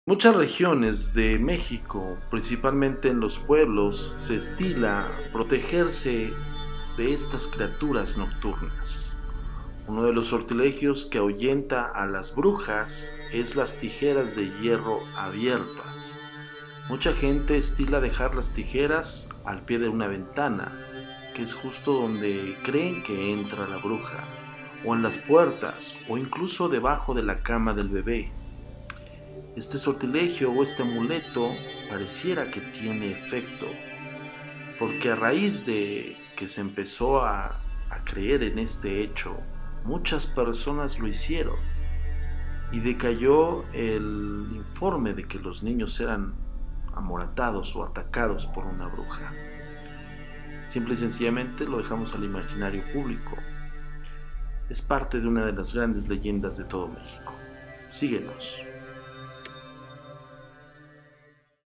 Brujas reales... "Creaturas nocturnas"
Sí, leíste bien! Creaturas son aquellas personas que con la vinculación con demonios familiares y demonios de jerarquía alta en comunión durante rituales pueden cambiar su comportamiento de humano a bestia...